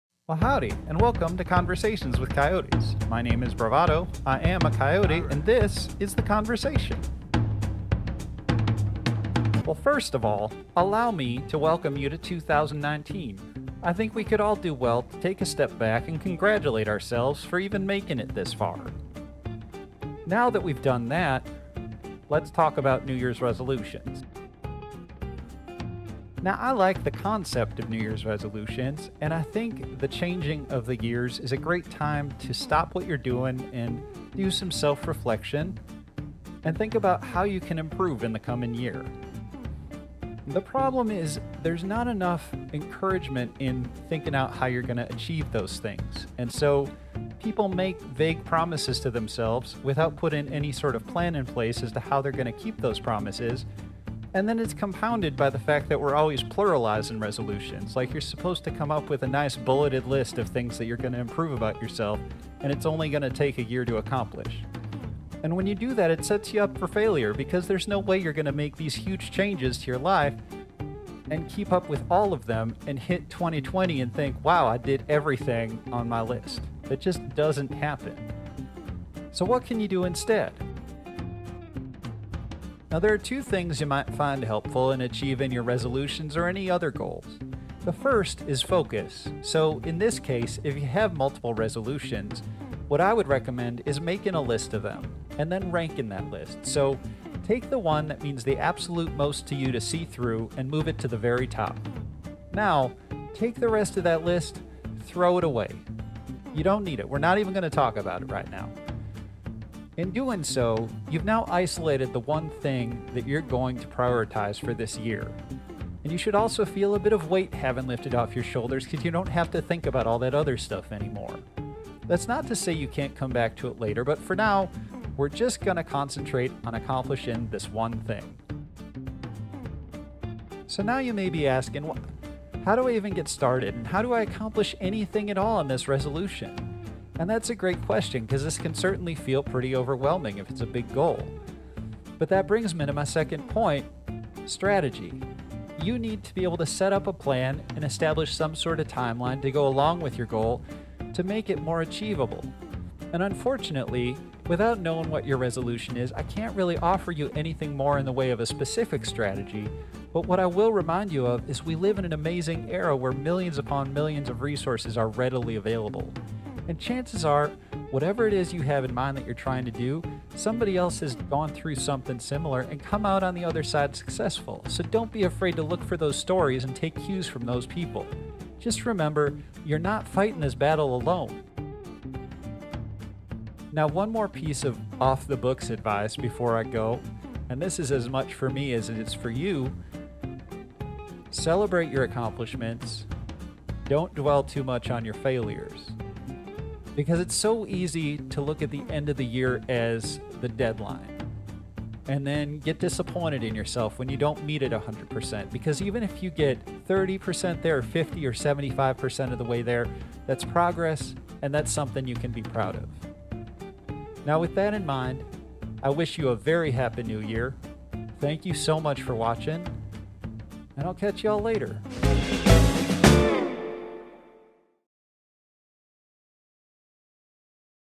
New Years Resolutions - Conversations with Coyotes, Episode 21
Hey did you make resolutions? Are you gonna keep them?